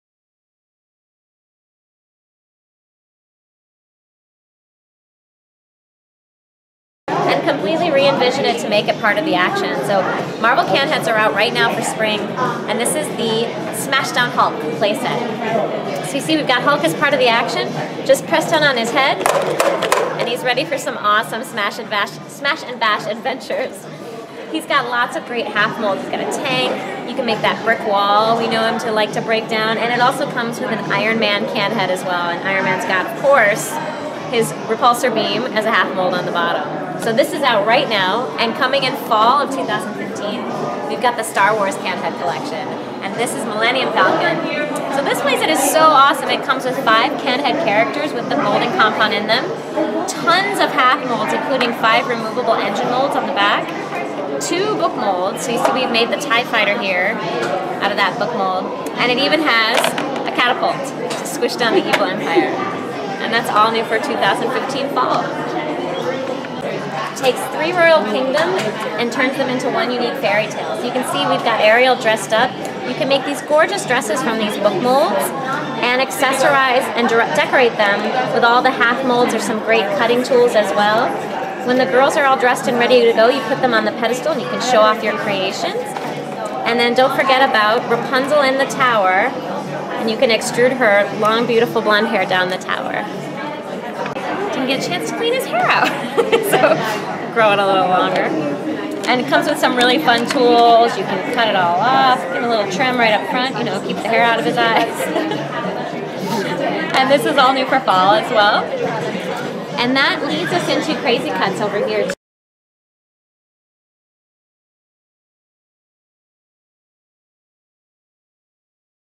Play Doh 2015 Products
Divina and Eddy get a chance to preview new 2015 Hasbro products at NYC Toy Fair. Check out new Play Doh Toys.